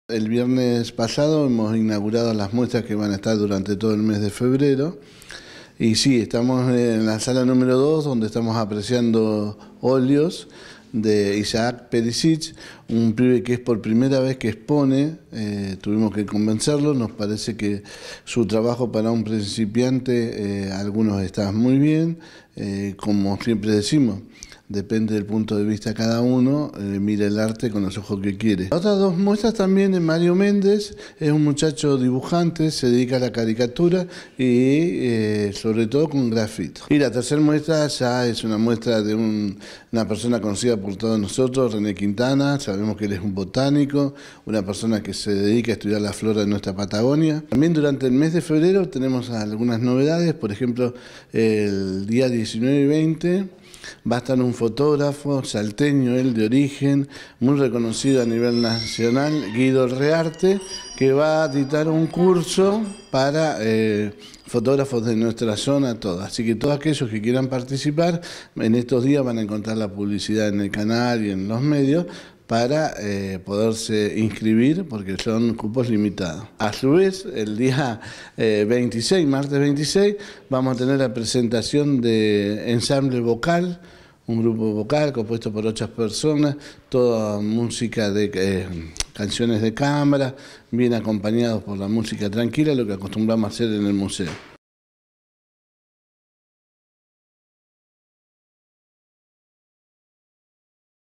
Propuestas del MAV
El Museo de Artes Visuales de Trelew a partir de esta semana cuenta con 3 exhibiciones de los artistas Mario Méndez, René Quintana e Isaac Pericich. Sus obras comprenden la caricatura, la fotografía y la pintura. Hablamos con Manuel López, dir. MAV